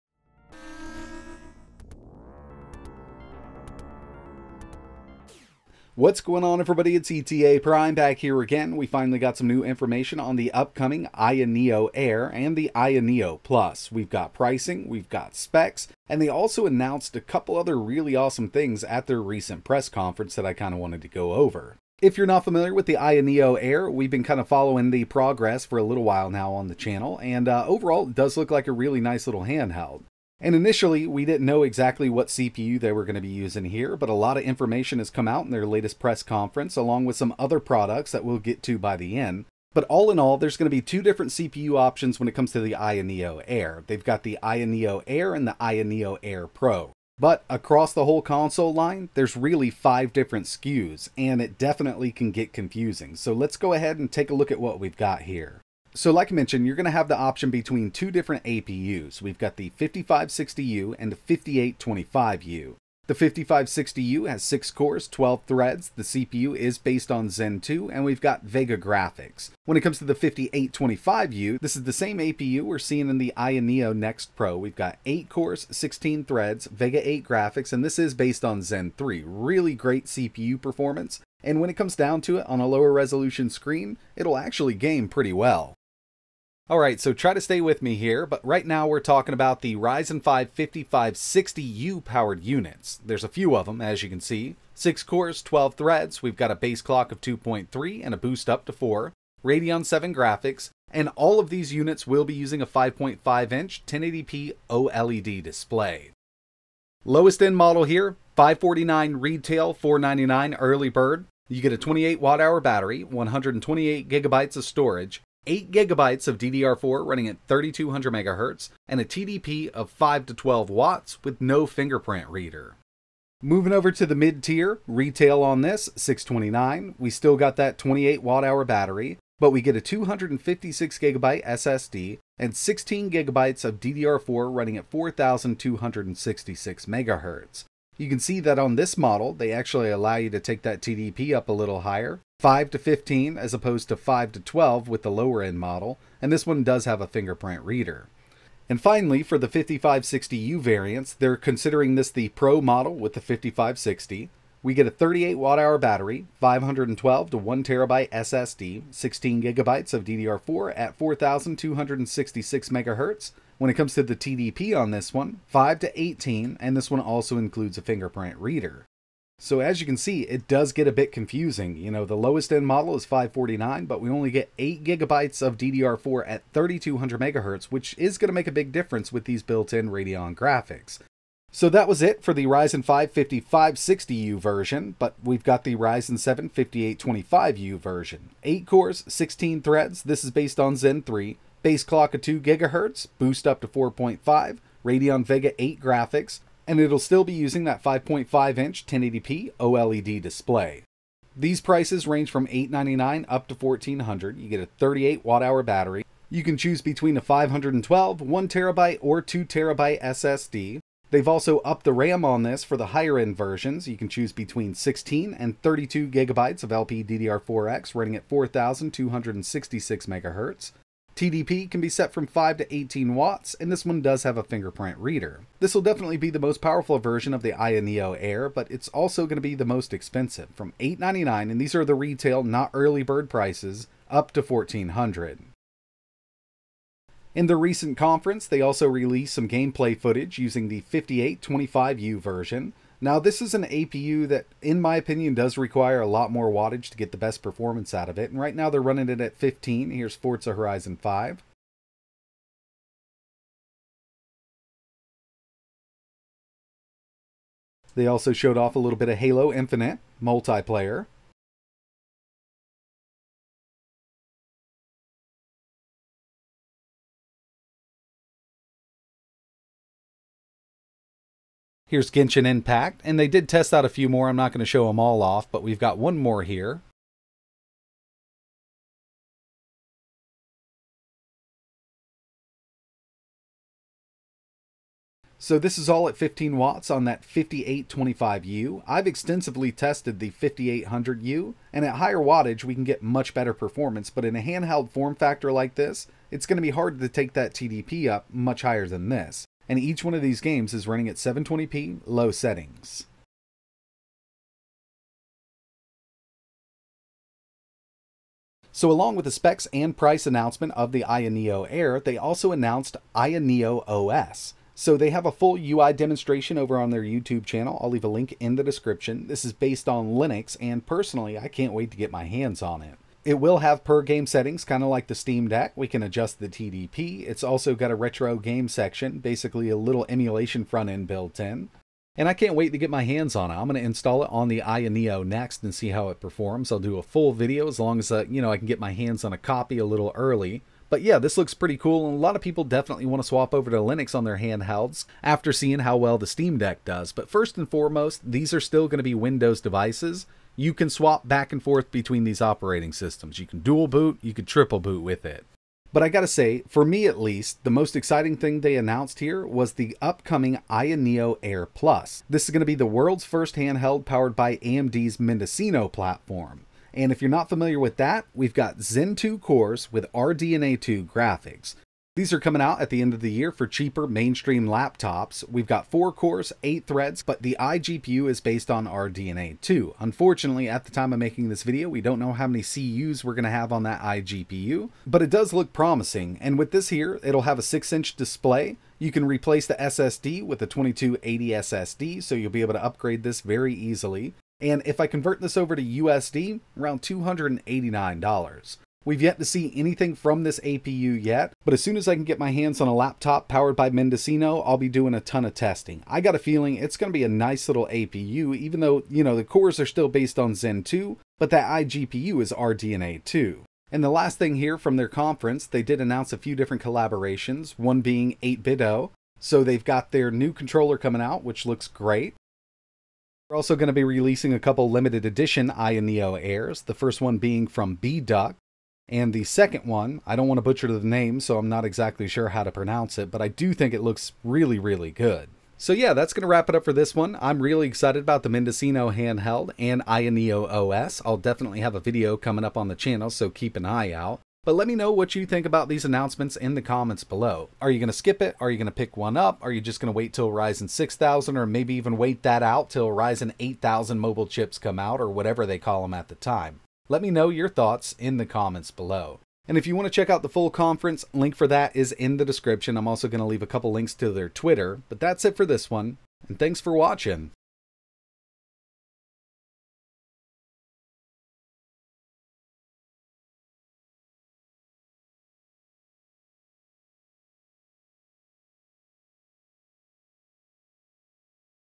A New $289 AMD Mendocino Hand-Held Is On The Way And AYA Neo Air Pro Specs, Price
AYA just Announced an upcoming handheld powered by AMD's new Mendocino APU and it is priced at $289 and
We’ve got new info like Specs Price and gaming benchmarks for the Aya Neo Air and Aya Neo Air Pro. Is it gonna be able to compete with the Steam Deck?
They also revealed Aya Neo OS and it is based on Linux!

ETAPRIME
12520 Capital Blvd Ste 401 Number 108